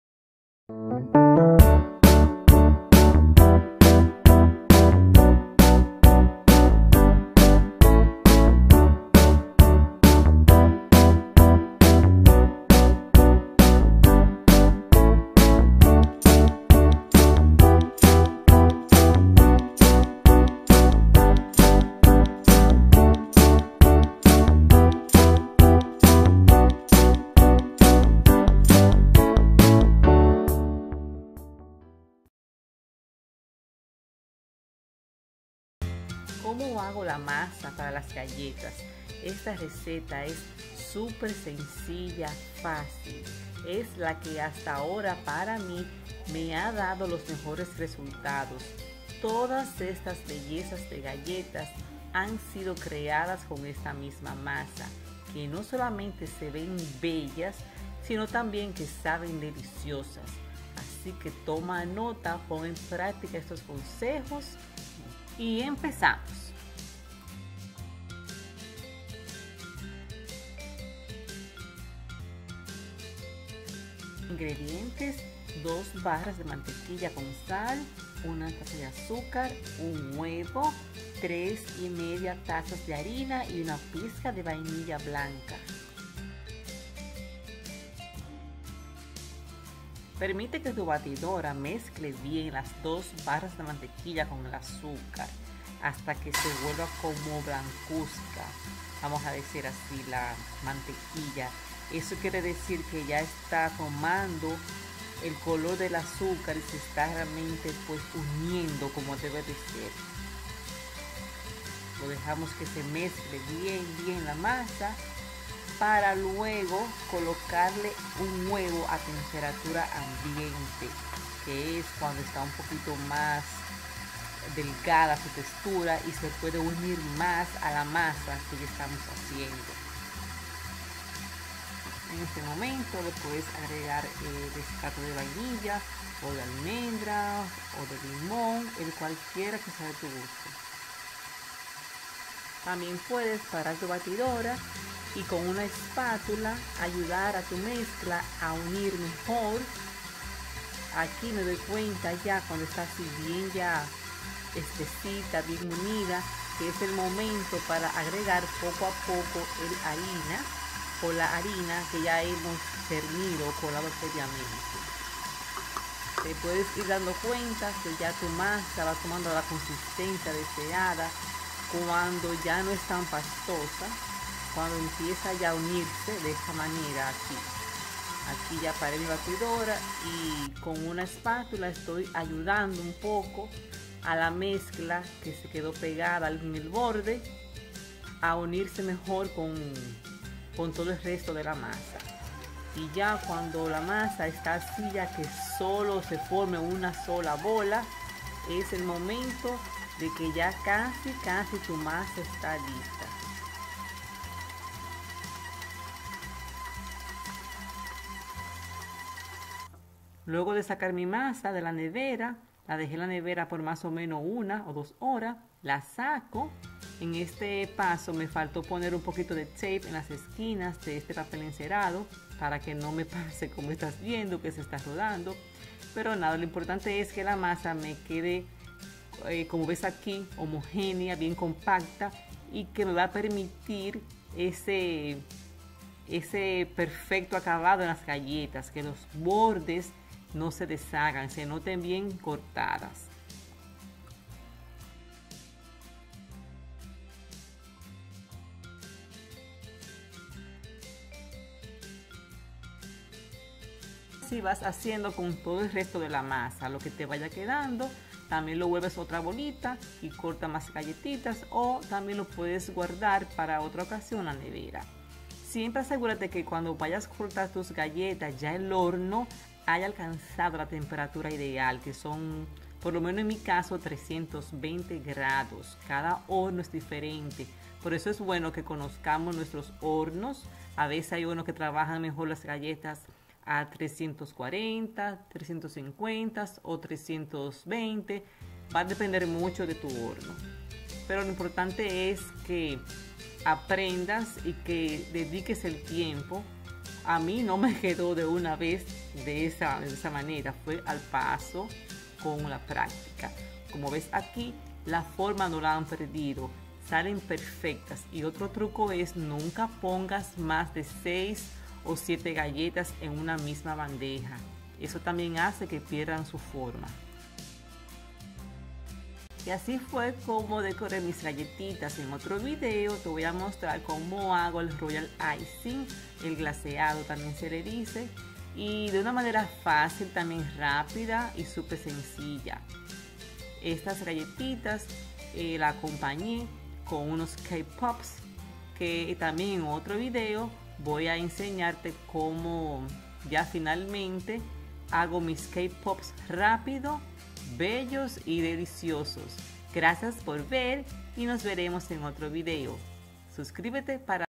RECETA DE GALLETAS FACIL Y CON POCOS INGREDIENTES ♥
Hola! aquí les dejo como yo hago mis galletas. Luego de probar muchas otras recetas y maneras esta es la que más uso. Quedan super deliciosas. Ojalá te pueda ayudar en algo.